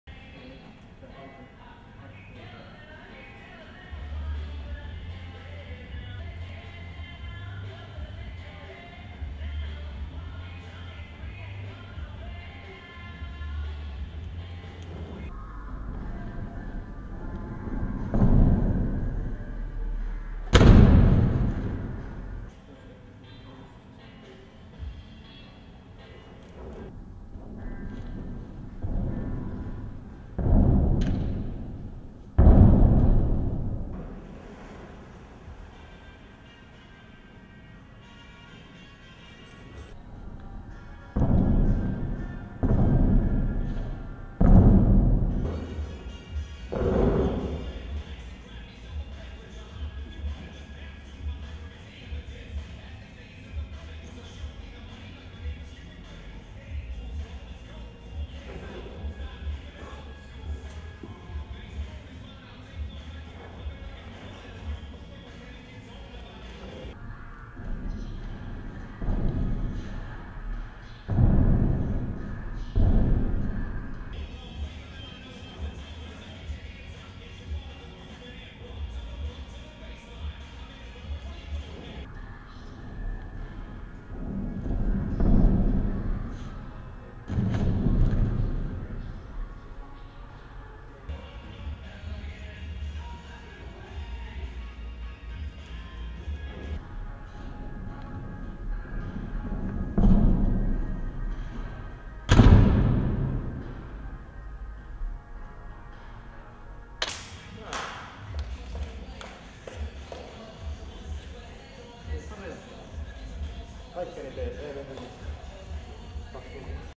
Sakari Vekki, 10.8.2013, Harjoituskilpailu, Gymnastics, Floor Exercise, FX, Finland
Valmistutumista Suomen MM-karsintaan 2013.
Jyväskylän  uusi jousikanveesi toimii hyvin.